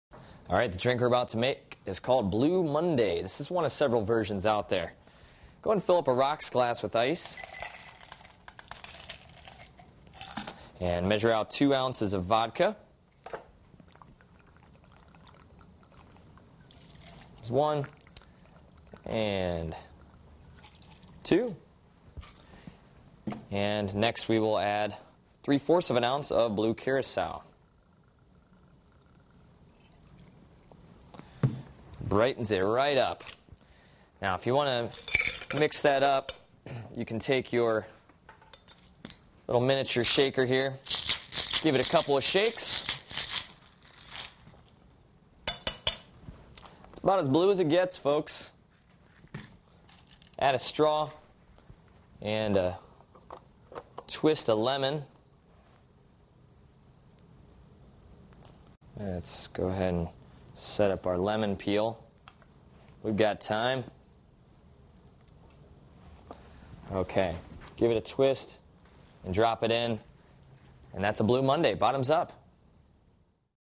How to Make the Blue Monday #3 Vodka Drink
How to Make the Blue Monday #3 Vodka Drink. Part of the series: Vodka Mixed Drinks. The Blue Monday #3 is a vodka-based mixed drink that's easy to make at home. Our professional bartender shows you how in the free video on mixed drinks.